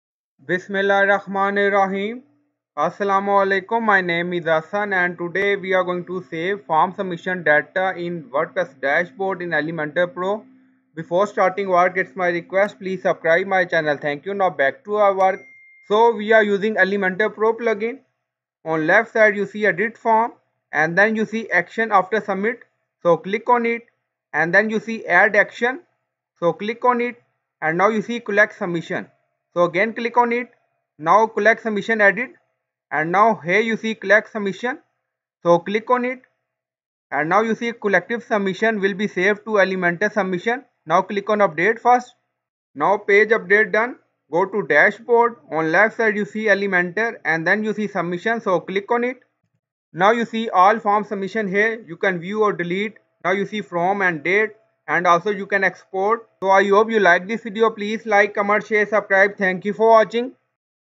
How to Save Form Submission Data in WordPress Dashboard in Elementor Pro
In this wordpress tutorial for beginners you will learn how to save / collect forms submission in wordpress dashboard using elementor pro website builder plugin.